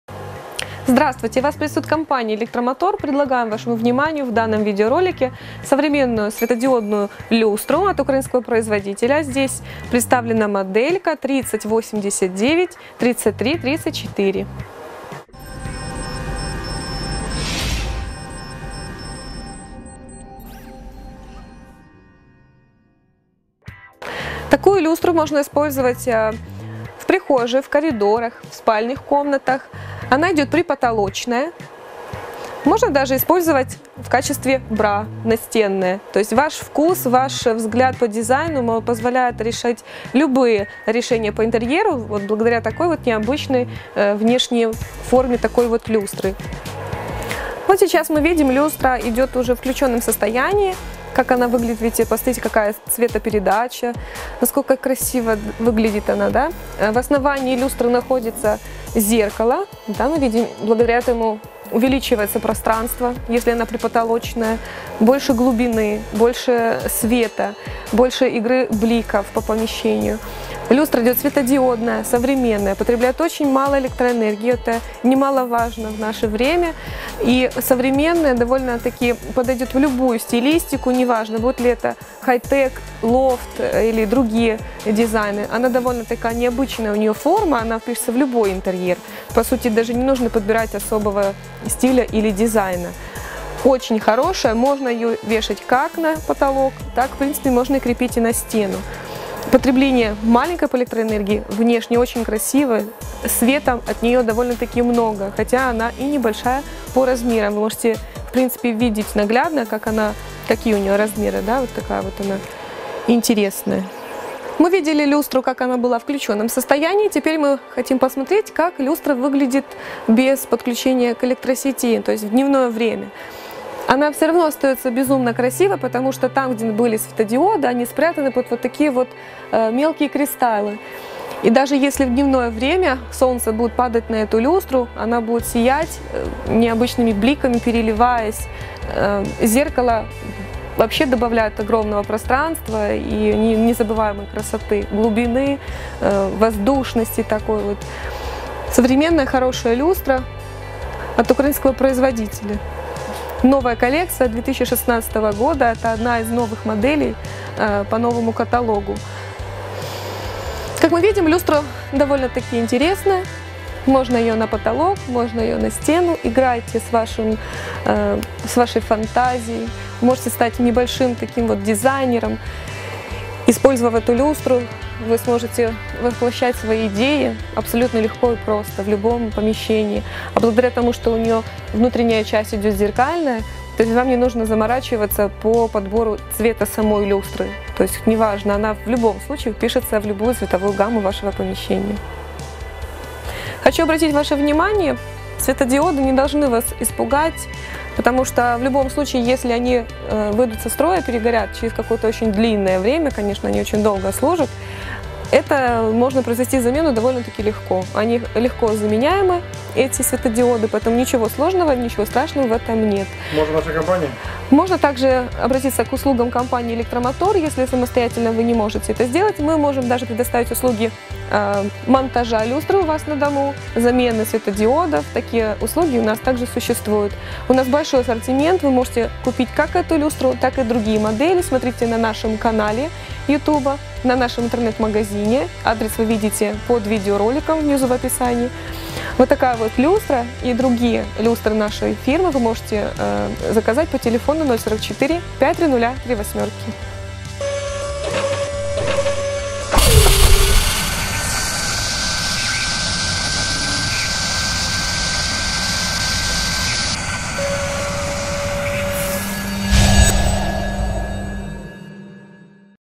Светодиодная Люстра Интеркласс 30893334
Люстра Интеркласс модель 30893334 - коллекция 2016 года. Компания Электромотор Киев. Контактный телефон 5000888.
Любые люстры Интеркласс со склада в Киеве